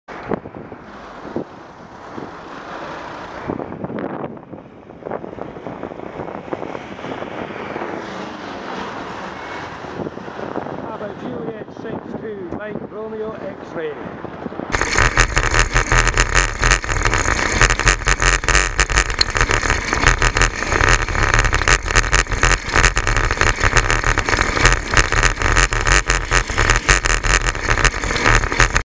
PJ62MRX pedestrian refuge
You all saw it, that pedestrian refuge ran into the middle of the road with no warning whatsoever.

Thursday 13th November 2014 approx 18:00